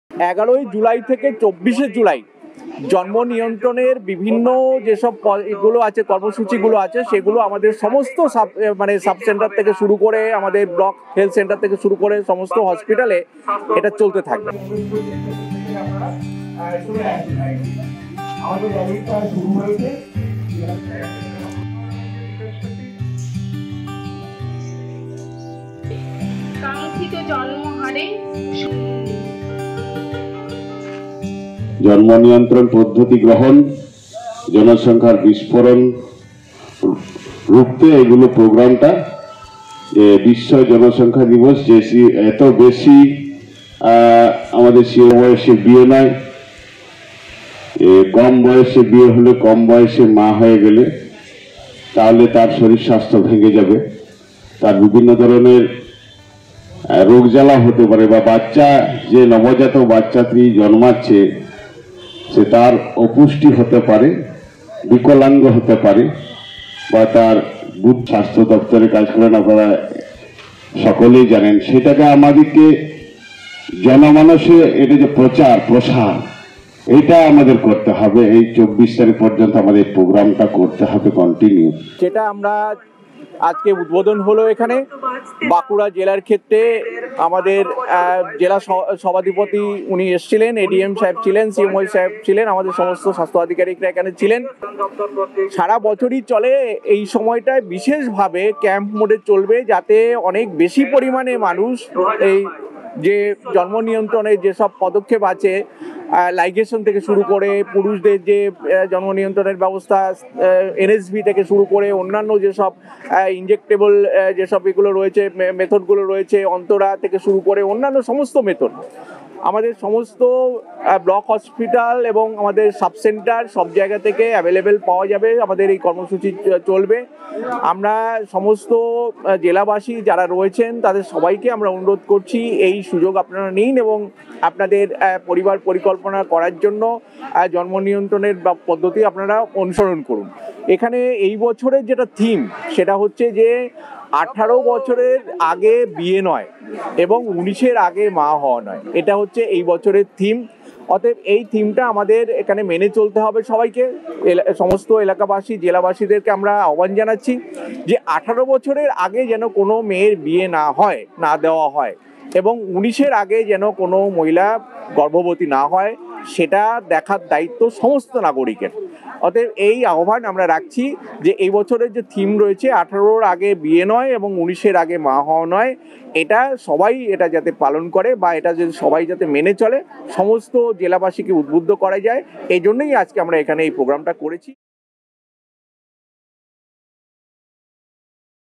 '১৮ র আগে বিয়ে নয়, ১৯ র আগে মা নয়' | Bankura | World Population Day | Ei Samay
'বিশ্ব জনসংখ্যা দিবস', জনসংখ্যা নিয়ন্ত্রণের অঙ্গীকার! ‘১৮ বছরের আগে বিয়ে নয়, ১৯ বছরের আগে মা নয়'। স্লোগানকে দিয়ে শুরু 'বিশ্ব জনসংখ্যা নিয়ন্ত্রণ পক্ষের' সূচনা।